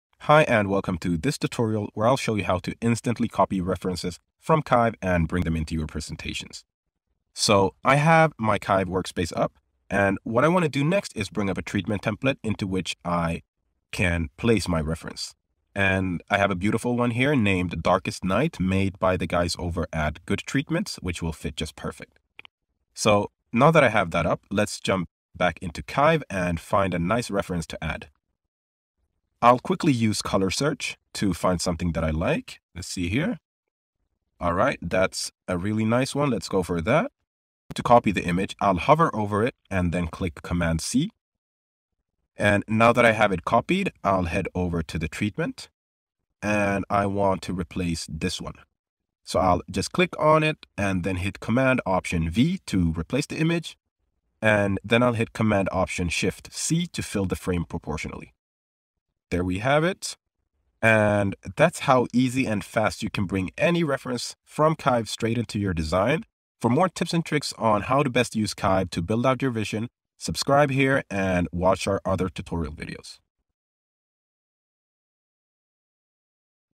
How to copy & paste images into your pitch deck
Learn how to create beautiful treatments, pitch decks and mood boards by instantly copying & pasting the perfect images from your Kive.